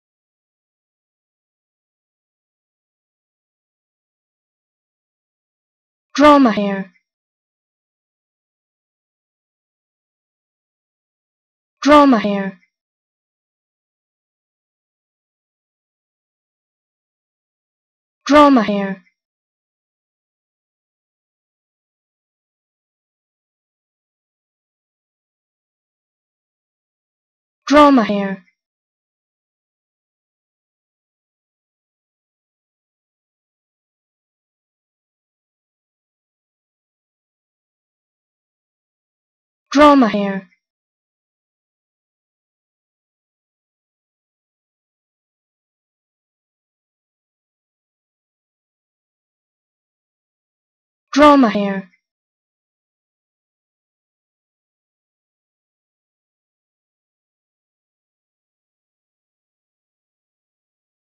How To Say Dromahair
Learn how to say Dromahair with EmmaSaying free pronunciation tutorials.